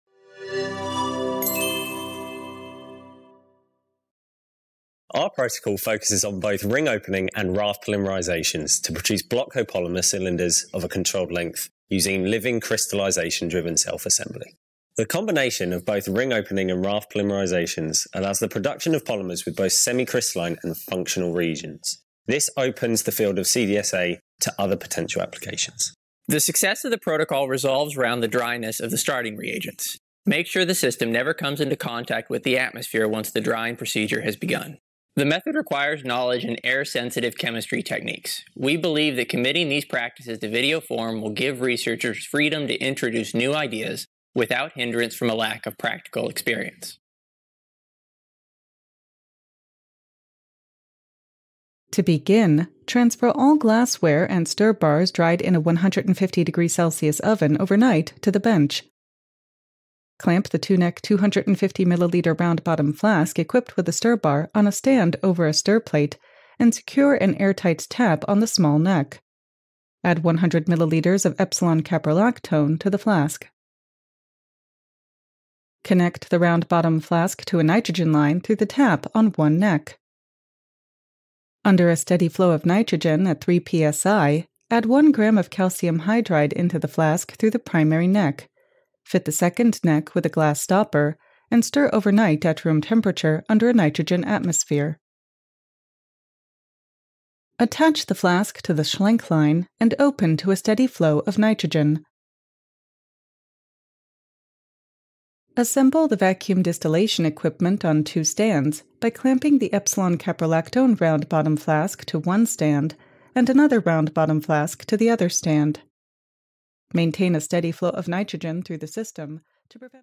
Monodisperse Cylindrical Nanoparticles: Crystallization-Driven Self-Assembly l Protocol Preview
Synthesis of Monodisperse Cylindrical Nanoparticles via Crystallization-driven Self-assembly of Biodegradable Block Copolymers -  a 2 minute Preview of the Experimental Protocol

Zachary Coe, Andrew Weems, Andrew P. Dove, Rachel K. O'Reilly
University of Birmingham, Department of Chemistry;

Crystallization-driven self-assembly (CDSA) displays the unique ability to fabricate cylindrical nanostructures of narrow length distributions. The organocatalyzed ring-opening polymerization of ε-caprolactone and subsequent chain extensions of methyl methacrylate and N,N-dimethyl acrylamide are demonstrated. A living CDSA protocol that produces monodisperse cylinders up to 500 nm in length is outlined.